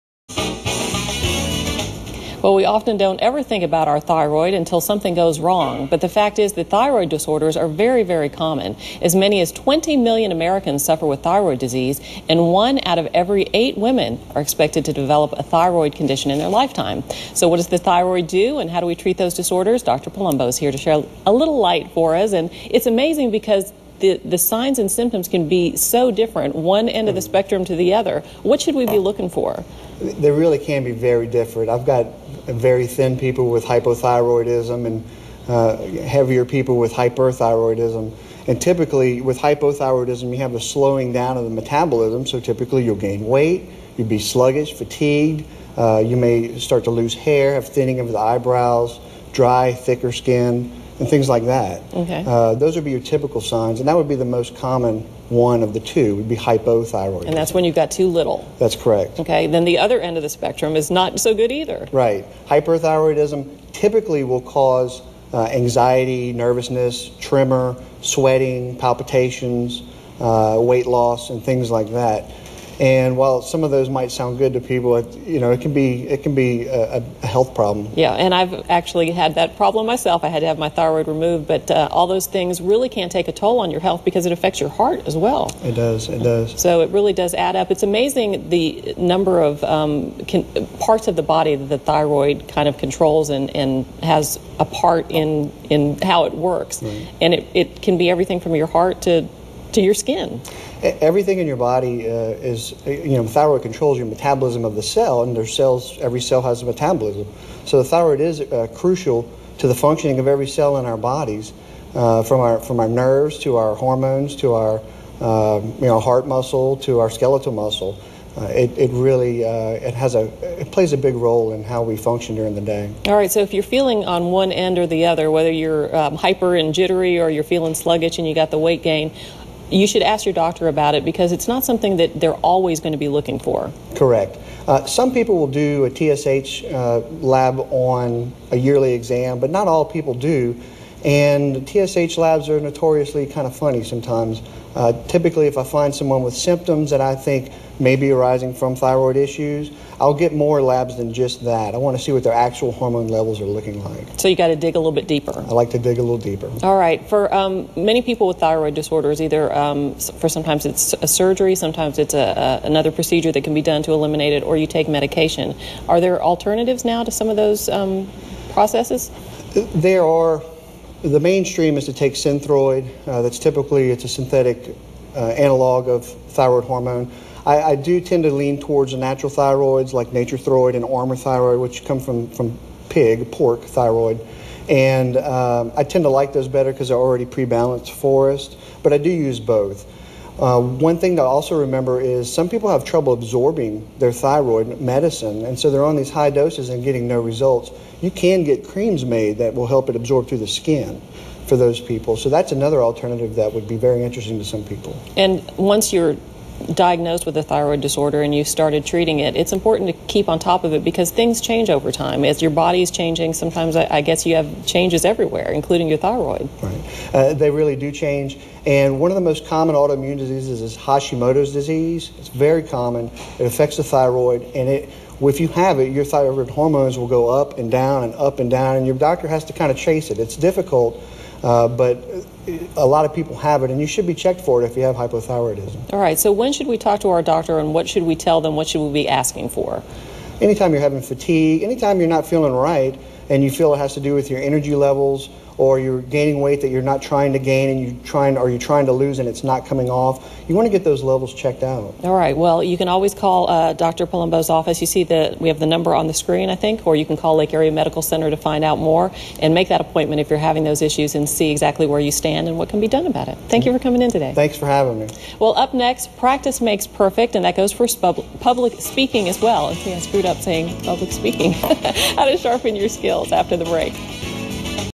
HOW TO TELL IF YOU HAVE THYROID ISSUES
DR. POLOMBO WITH LAKE AREA MEDICAL CENTER SHEDS SOME LIGHT ON A VERY COMMON PROBLEM.